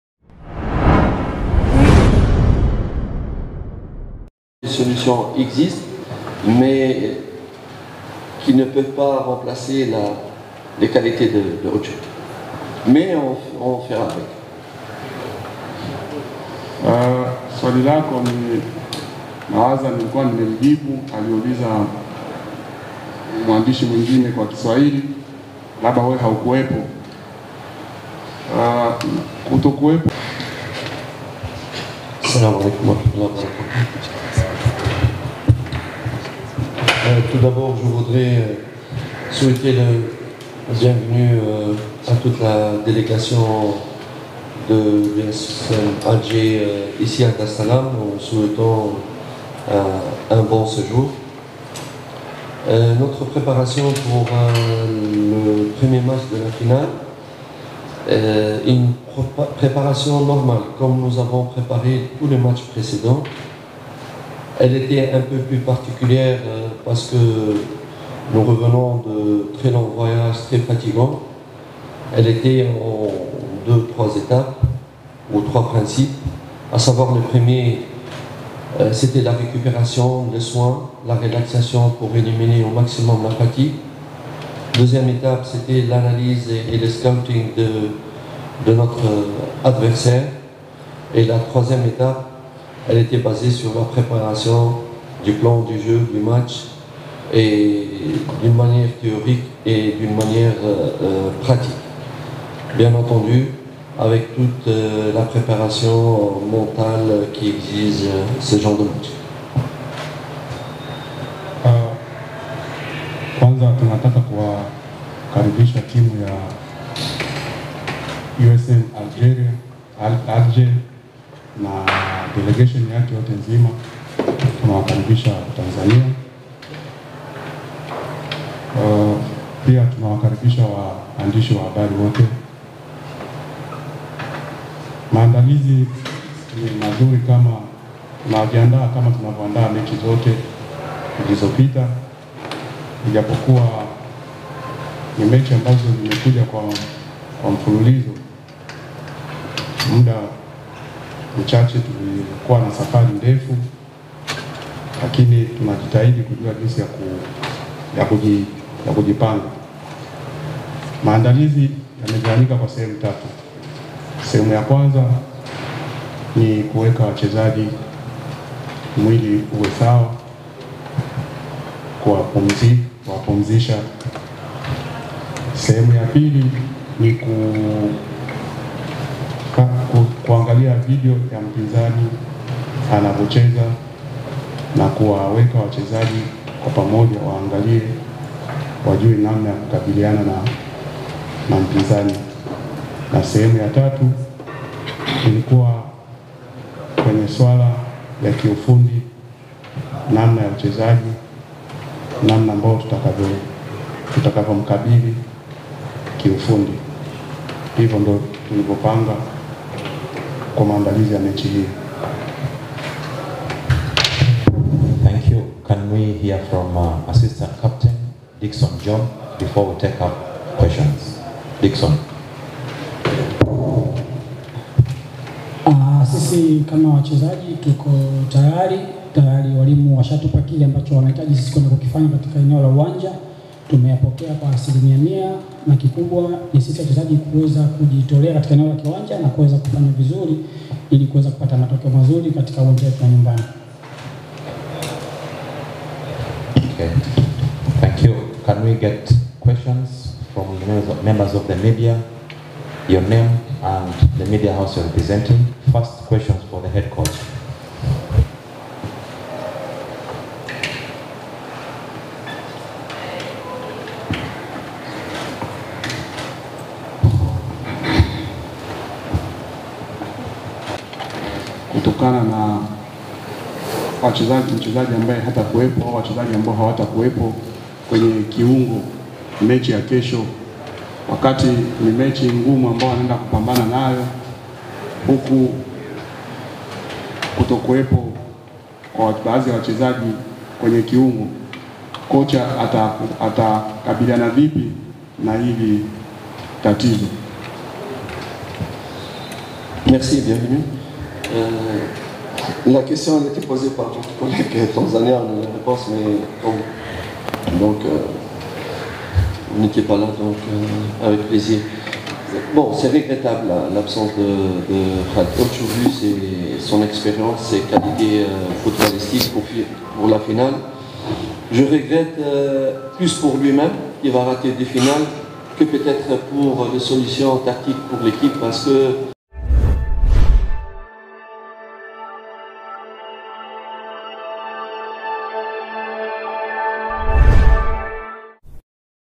🔴live_KIMENUKA! PIGO ZITO! AUCHO OUT YANGA, KOCHA YANGA ANATANGAZA KIKOSI UMEONA WATAKAOANZA KESHO?
WAMEISHAAA! AUCHO HAYUPO YANGA? KOCHA YANGA ANATANGAZA KIKOSI CHA KESHO NI BALAA! UMEONA ALICHOSEMA?